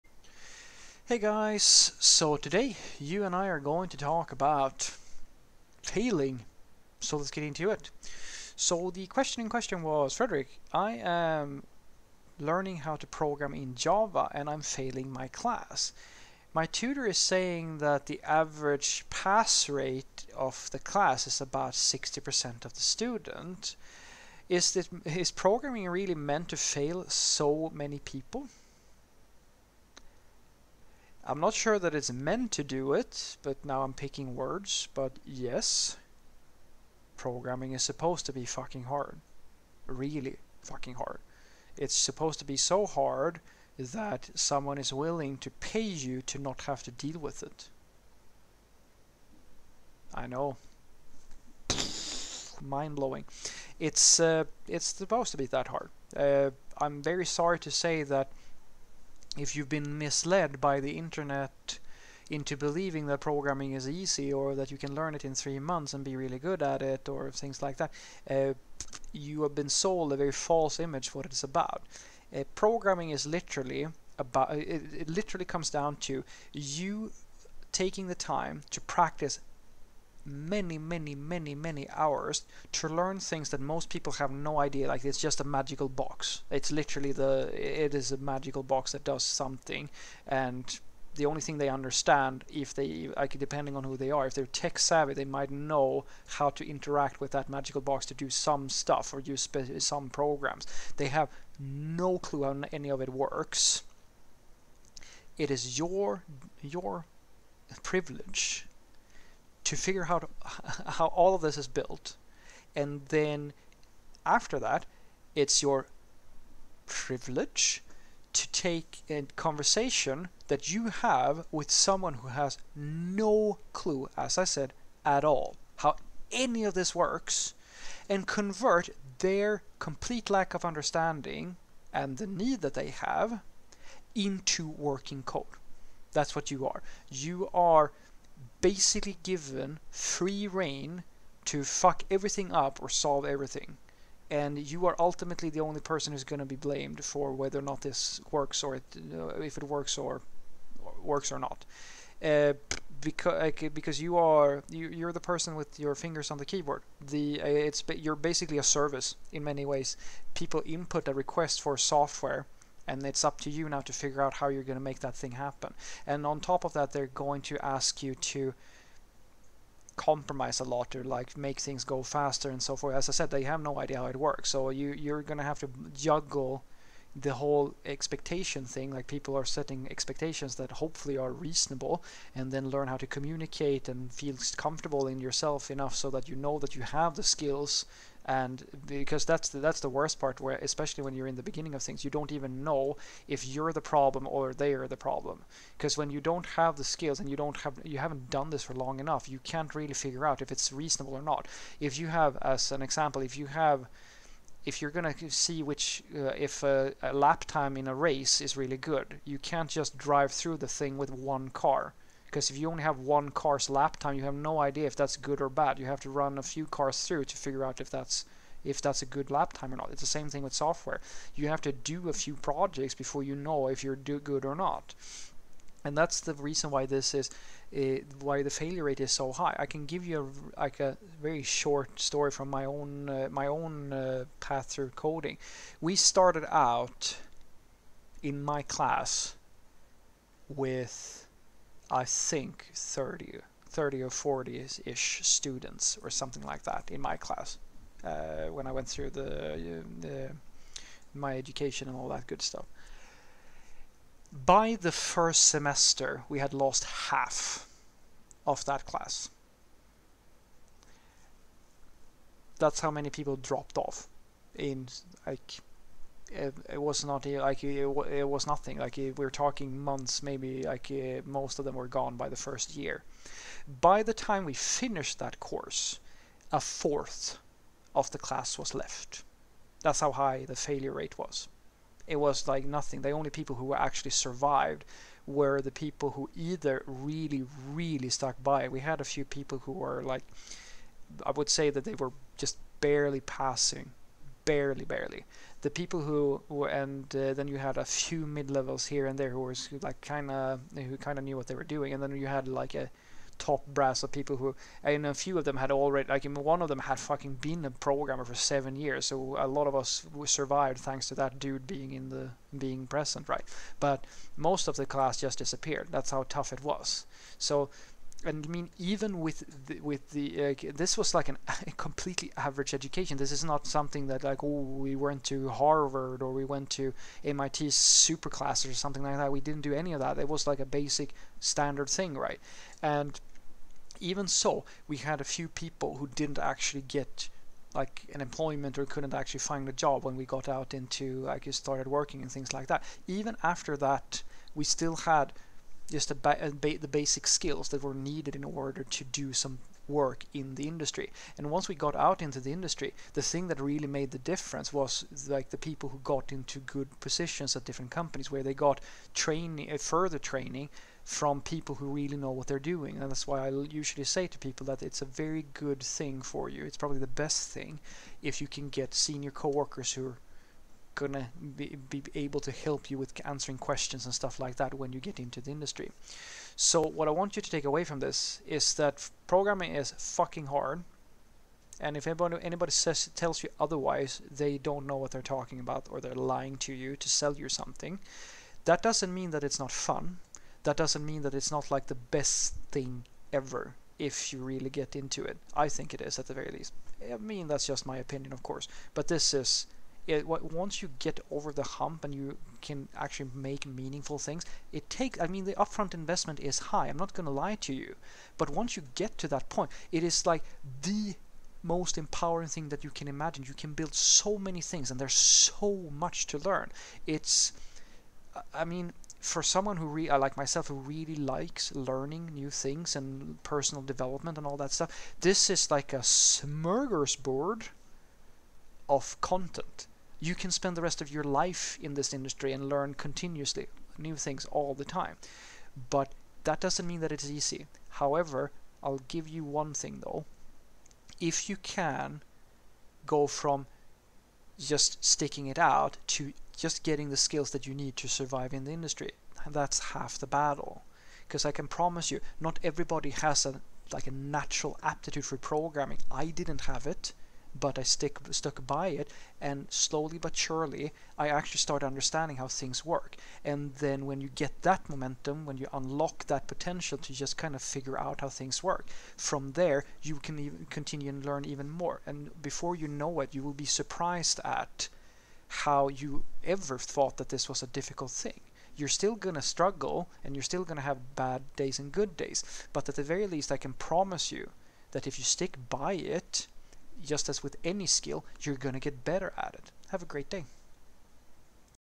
Is programming supposed to be this hard?
In "Is programming supposed to be this hard?" I answer a programming question.